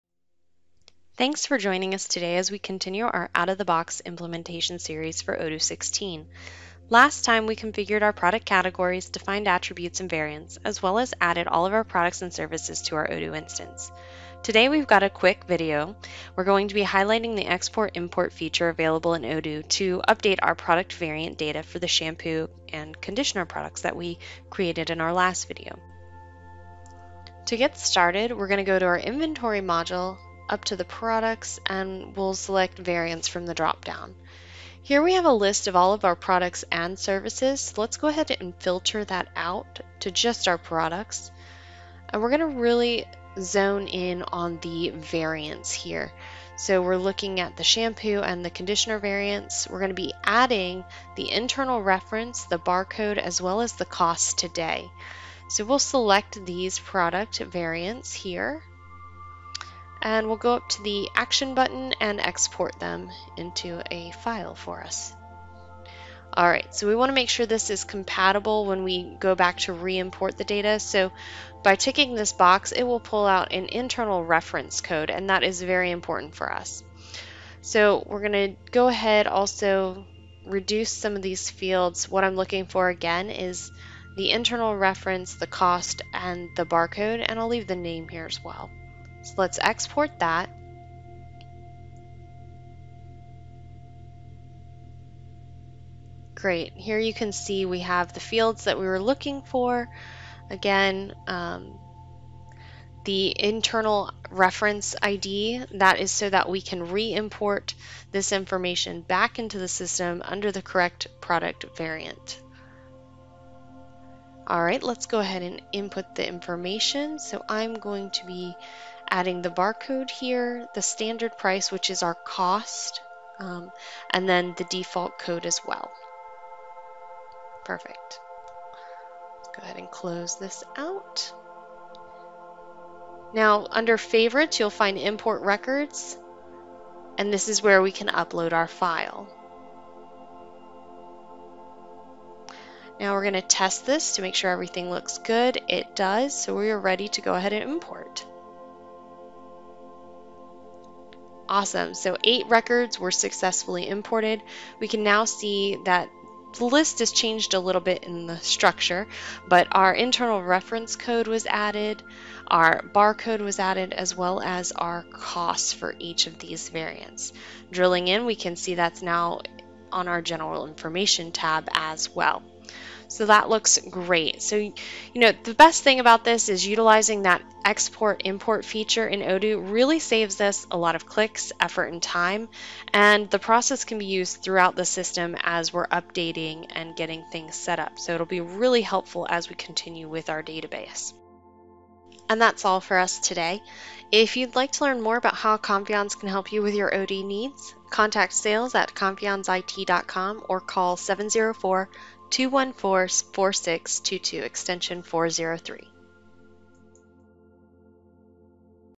How To Import/Export Data Using Odoo ERP | Odoo 16
We have a quick video today. We are going to highlight the export/import feature available in Odoo to update our product variant data for the Shampoo and Conditioner products that were created in our last video. Rather than going into each product form and manually updating, we are going to save some time and effort by exporting the data, updating it in a spreadsheet, and then importing the updated data back into our Odoo database.

Our office 🏢
Confianz Global Inc
5550 77 Center Dr #135, Charlotte,
NC 28217, United States